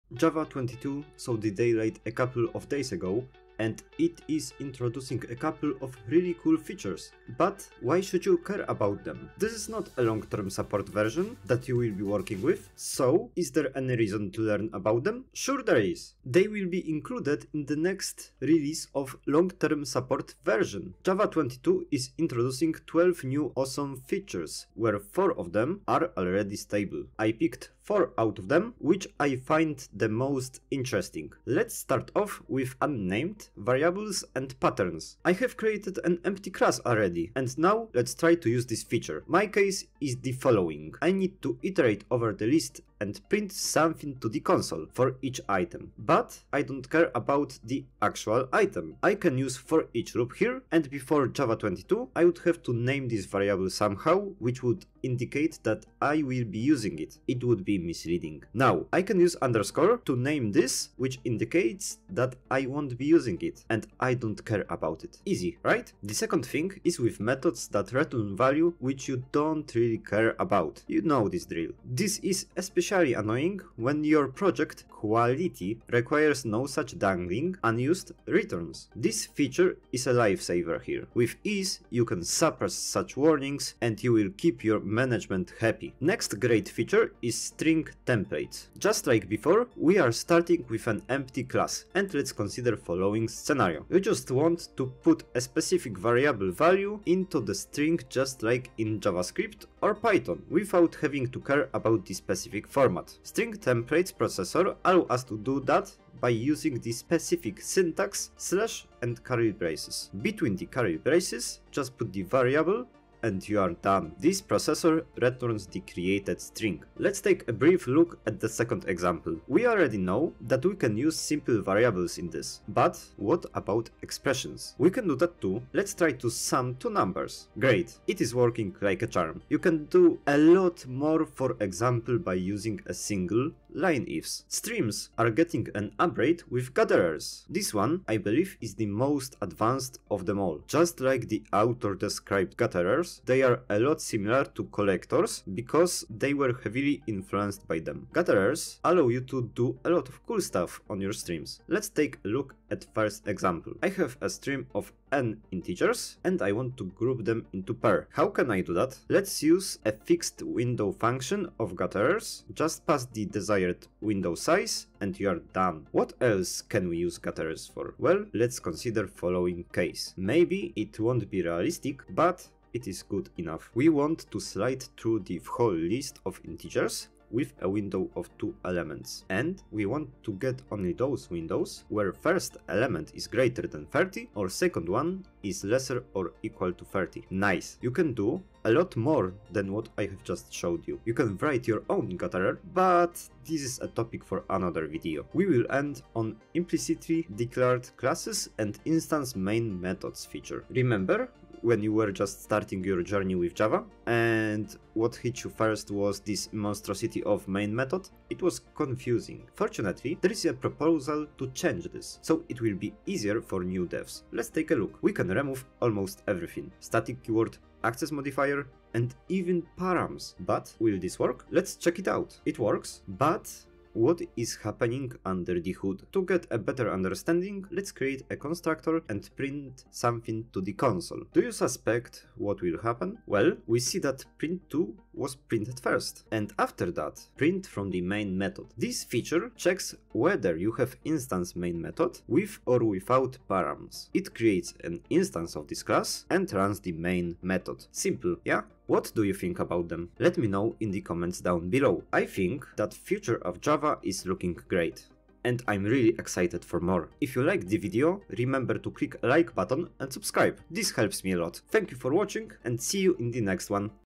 Java 22 is FINALLY HERE! And I love it!
Discover the latest features and improvements in Java Development Kit (JDK) 22! In this short video, we'll cover 4 out of 12 most interesting new updates and enhancements to exciting innovations and language changes introduced in 2024. We will explore the changes in variables, Strings, Streams and Instance Main Methods! Gain a deeper understanding of the evolving world of Java technology. Whether you're a seasoned developer or just starting out, this video is an essential resource for staying up-to-date with the ever-changing landscape of Java development.

You like my videos?